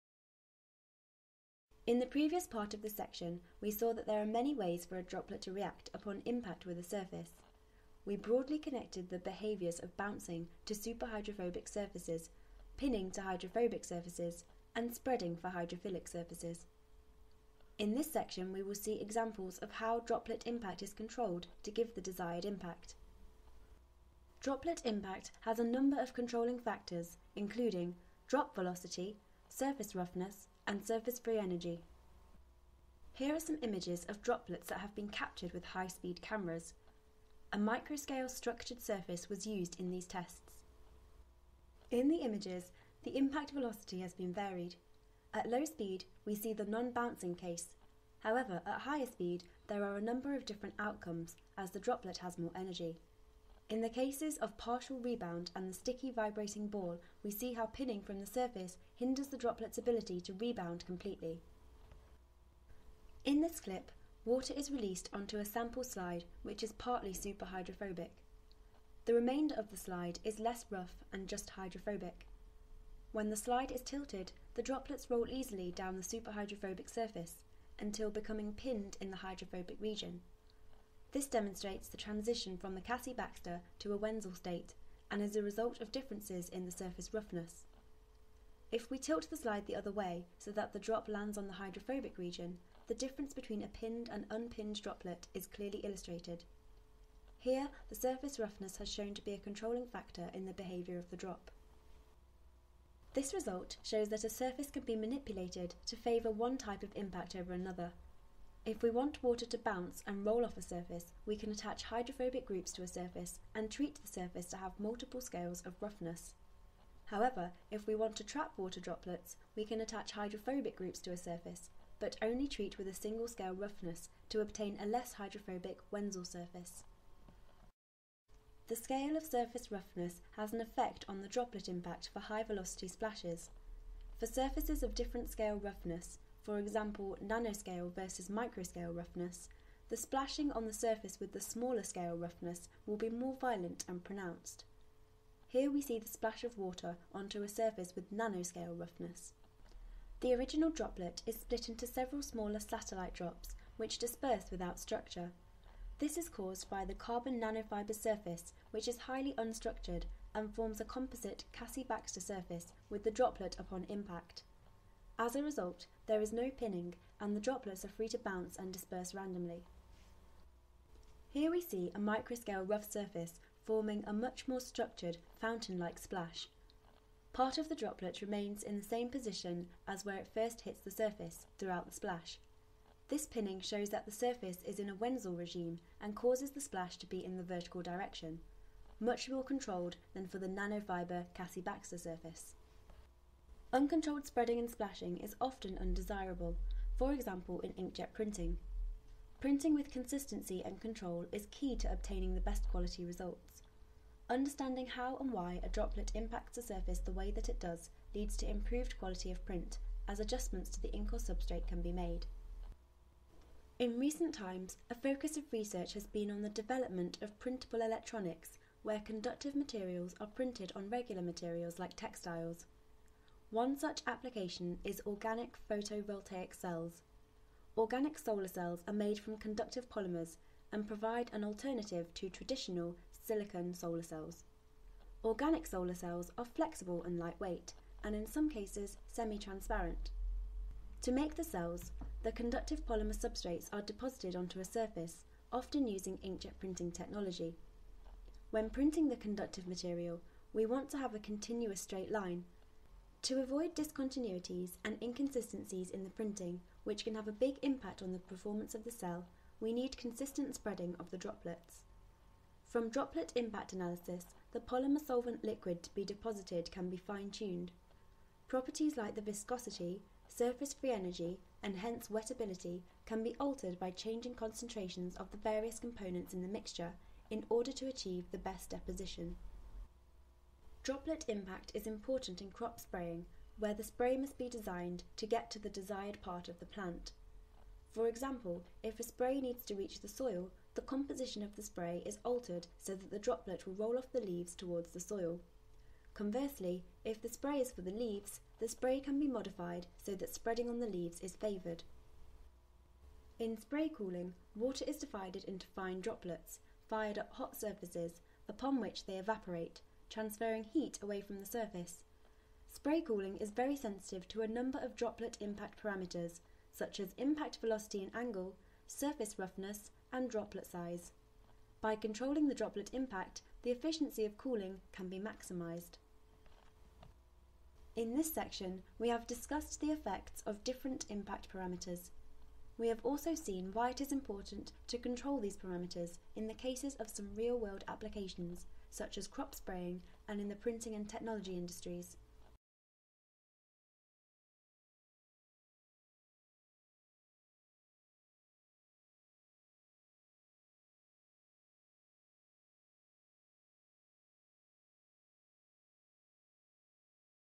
Functional Surfaces A5 - Droplet Impact (Advanced)
What are the controlling factors that determine droplet impact? We take a look and discuss how surfaces and the liquids being deposited can be manipulated to obtain the desired droplet impact.
The second part of section 6 in this series of online video primers teaching about the behaviour of water on surfaces. This video is designed for ages 18+ (Undergraduate level).